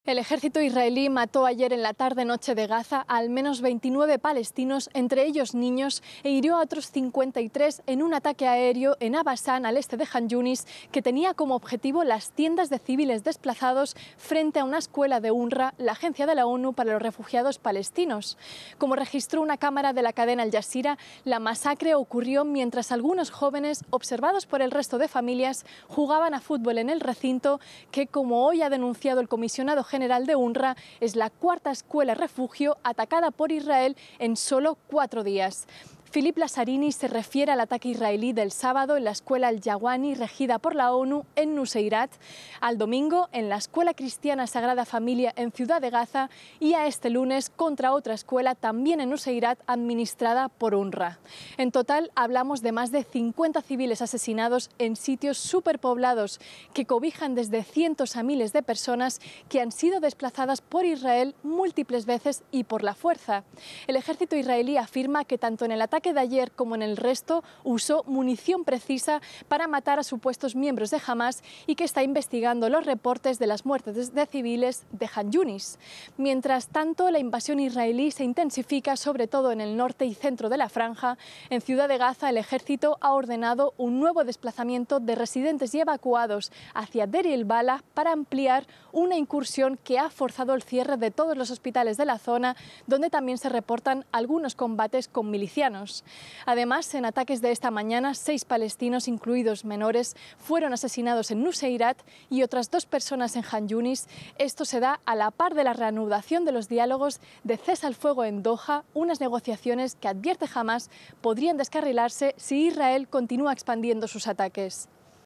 Informe desde Jerusalén: UNRWA condena cuatro ataques israelíes a escuelas en cuatro días en Gaza
Por cuatro días consecutivos, Israel ha atacado varias escuelas refugio en el territorio palestino. En el ataque del 9 de julio murieron al menos 29 personas y al menos 53 resultaron heridas; según un video del medio 'Al Jazeera', la masacre ocurrió cuando algunos jóvenes jugaban fútbol acompañados por sus familias. Nuestros corresponsales Federico Cué Barberena y Janira Gómez Muñoz nos contaron los detalles de esta situación.